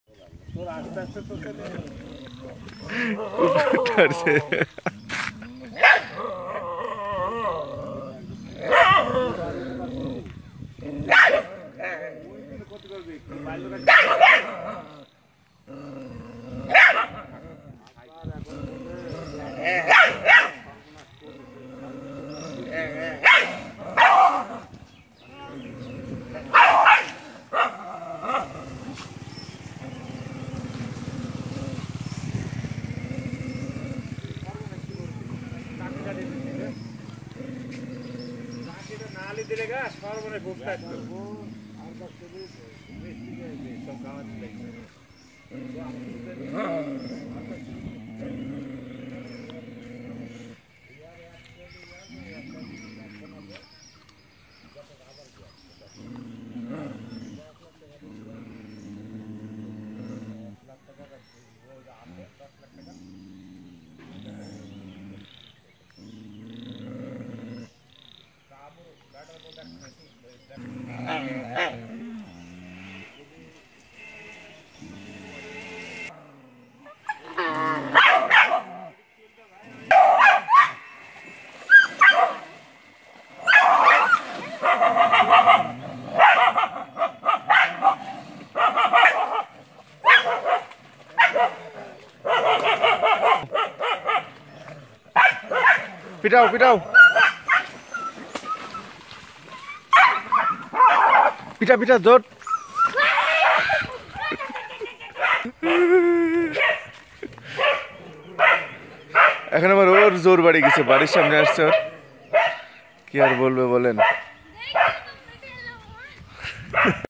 কুকুরের যুদ্ধ!!! 😎😎😁 | এক কুকুর আরেক কুকুরকে পানিতে নামায় ছাড়লো ! Dog Fight In Water.
This is dog fighting video in water place. I hope you all guys enjoy this video.
Thanks